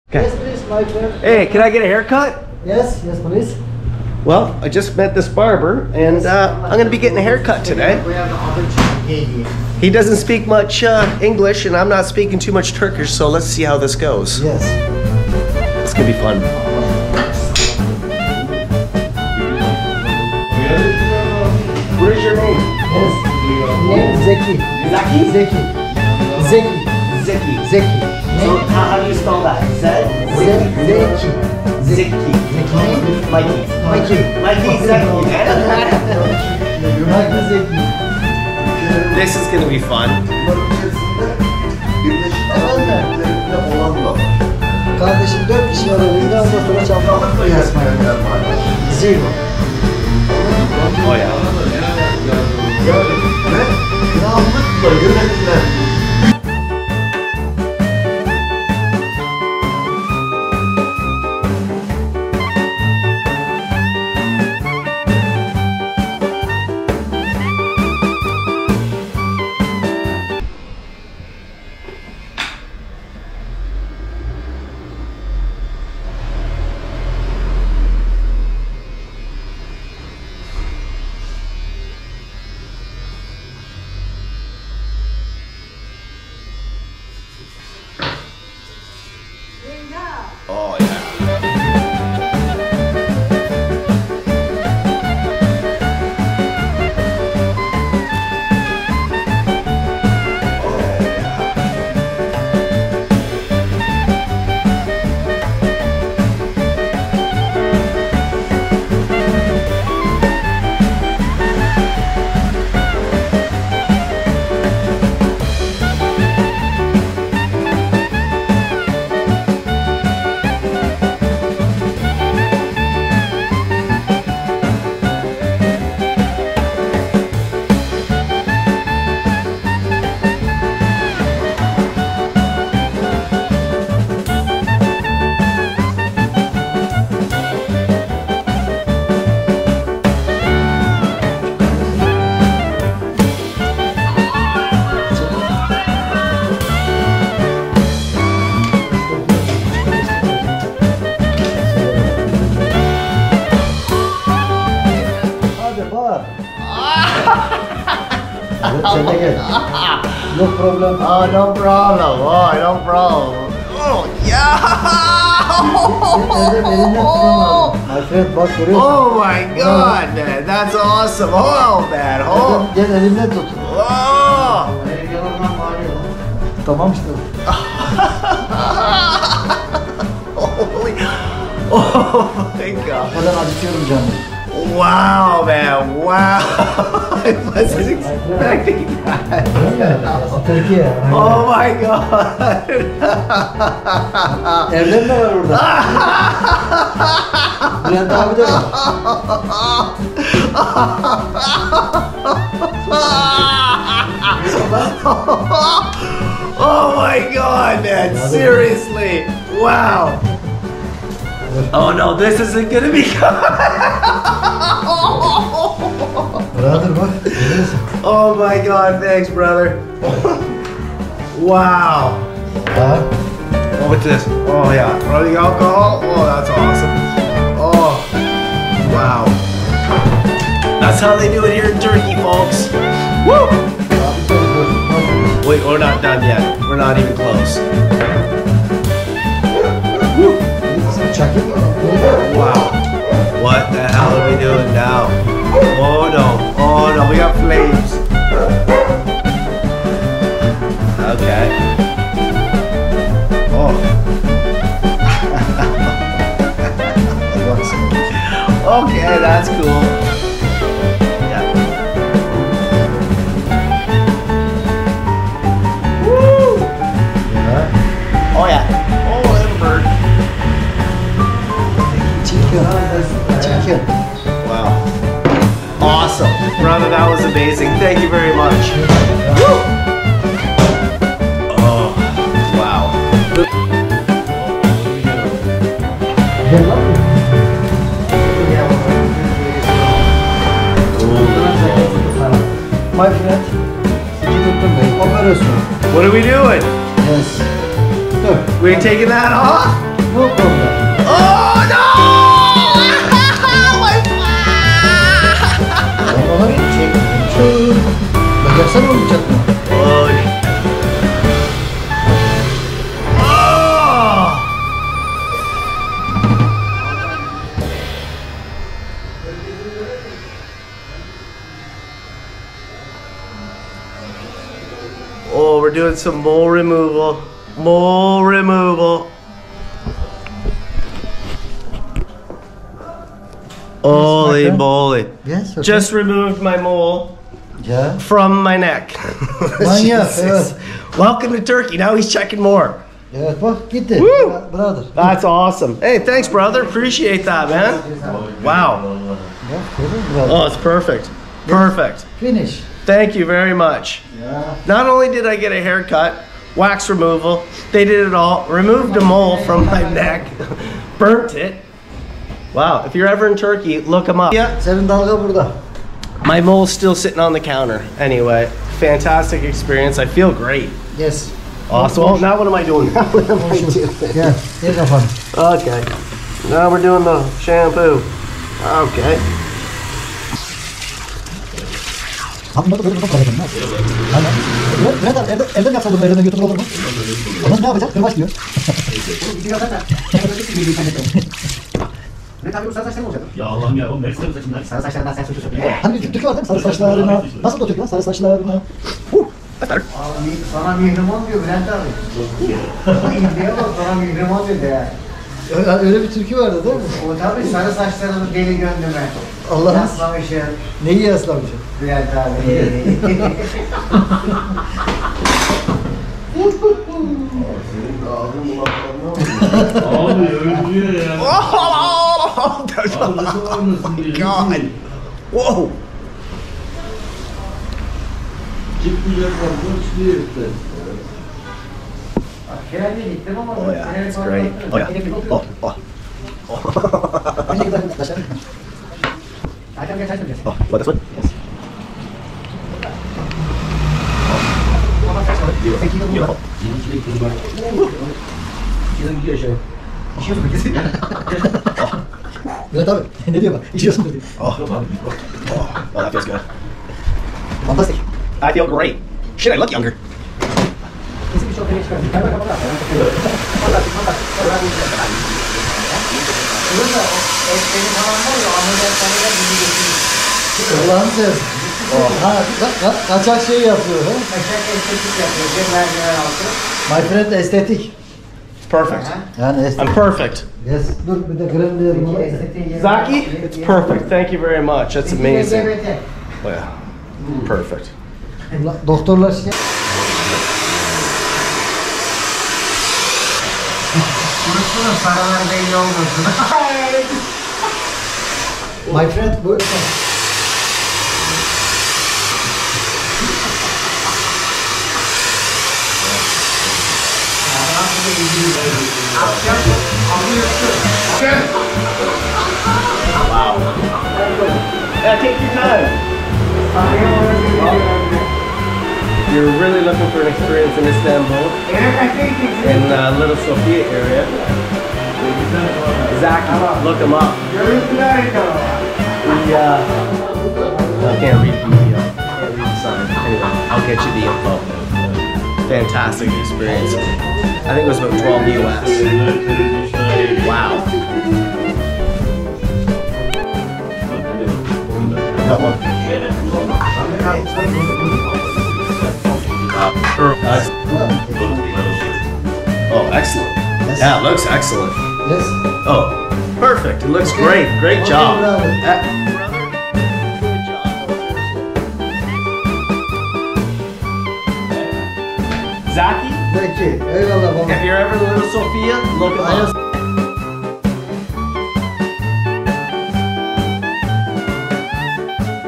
First BARBER SHOP EXPERIENCE In Istanbul TURKEY @Finding-Fish#barbershop
I walk in expecting a quick haircut but end up having not only my haircut but waxing of my nostrils and removal of a neck mole!!
Travel the Streets Of Instanbul Turkey to Find a Barber
His name is Zeki!!! Crazy Zeki Great Barber and Great Guy